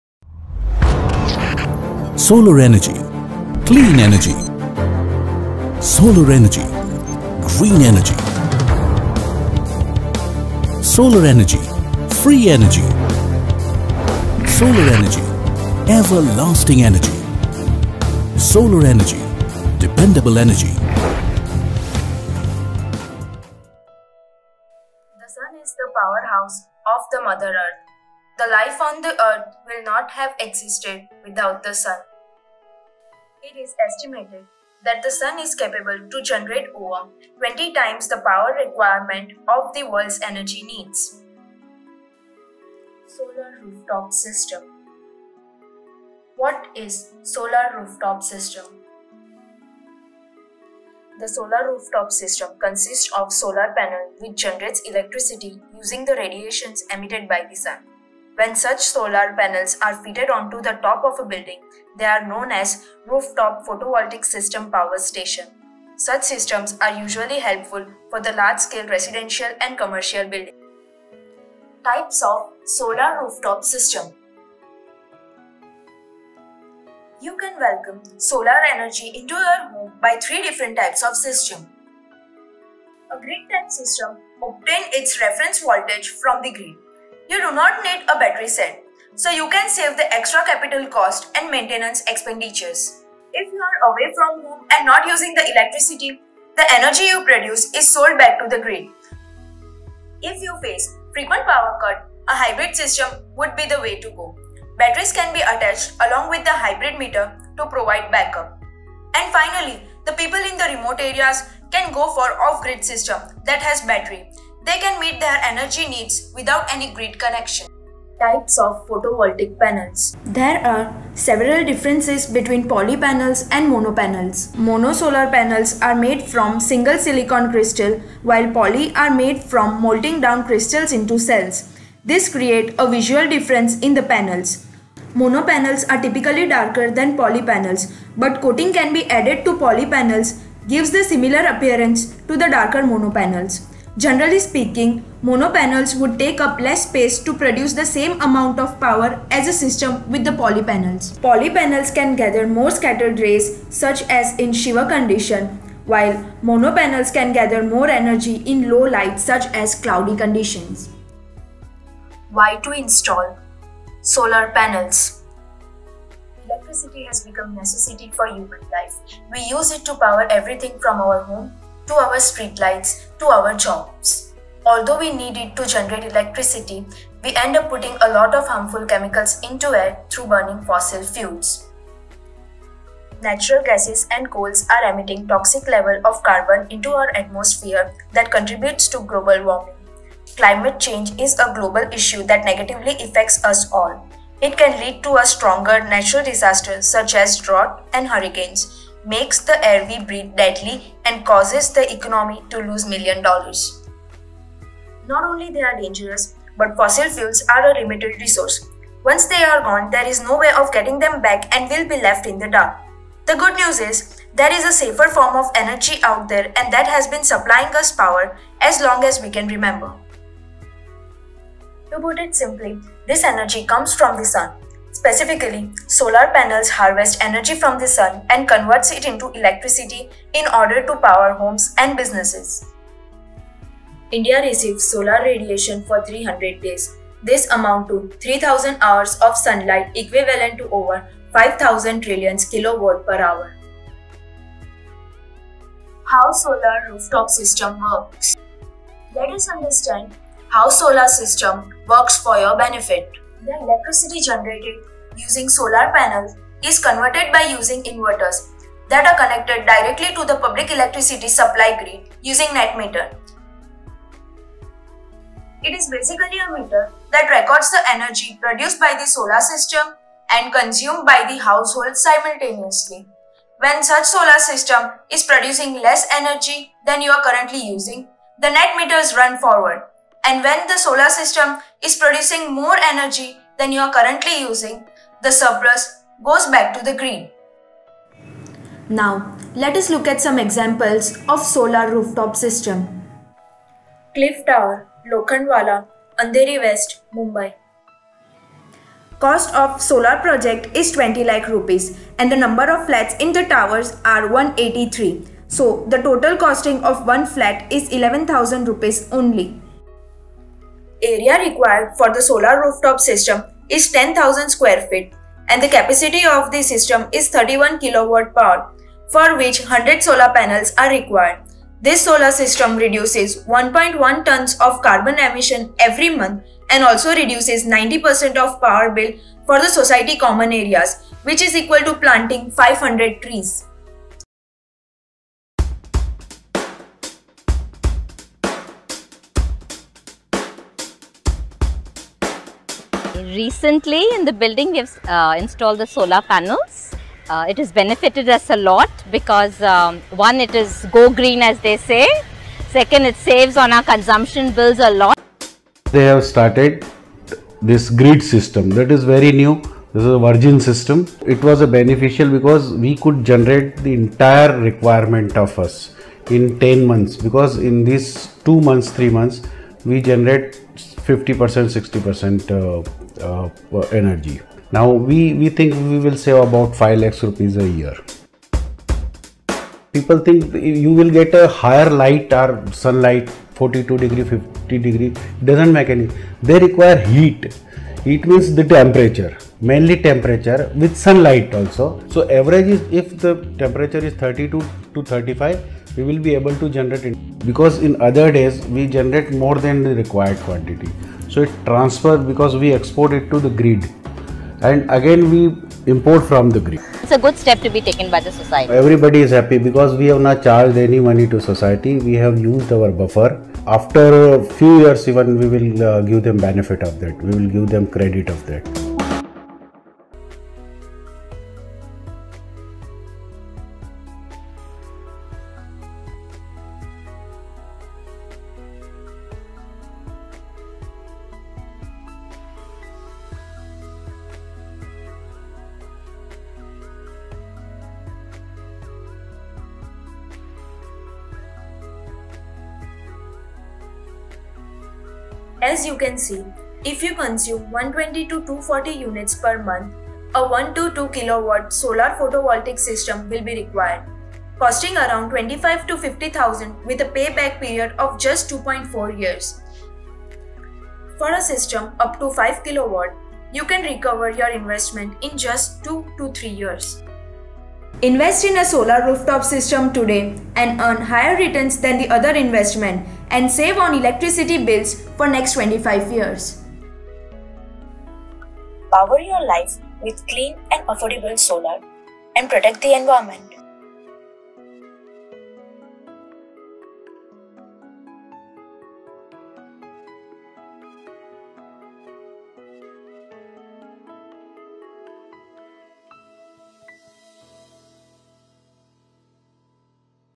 Solar Energy Awareness I English
The video sheds light on the abundant potential of solar energy, showcasing its ability to generate clean and sustainable electricity. Discover the various types of systems available, including grid-type, hybrid, and off-grid, while exploring the advantages of poly and mono solar panels.
The video goes beyond technical aspects and highlights the pressing environmental impact of fossil fuels, emphasizing the need to transition to renewable energy sources. Real-life examples illustrate the tangible benefits of solar rooftop systems, including significant cost savings and reduction in carbon emissions.